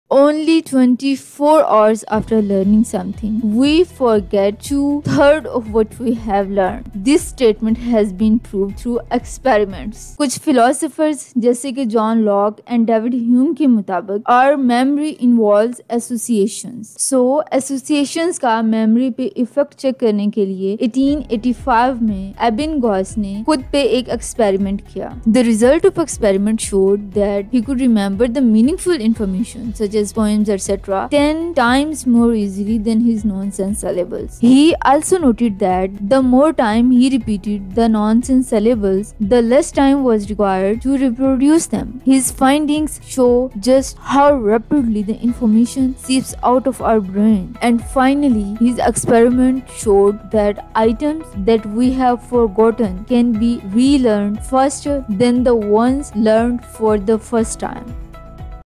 Ebbinghaus’s Memory Experiments: 24 HOURS AFTER LEARNING SOMETHING, WE FORGET TWO-THIRDS OF IT
24 HOURS AFTER LEARNING SOMETHING, WE FORGET TWO-THIRDS OF IT
In 1885, Hermann Ebbinghaus became the first psychologist to systematically study learning and memory by carrying out a long, exhausting experiment on himself.
In this video, I have discussed the results of the Ebbinghaus memory Research.

WHO AM I:
I'm Nafeesa, working as a Final Year Mphil Psychology Student at GC University Lahore.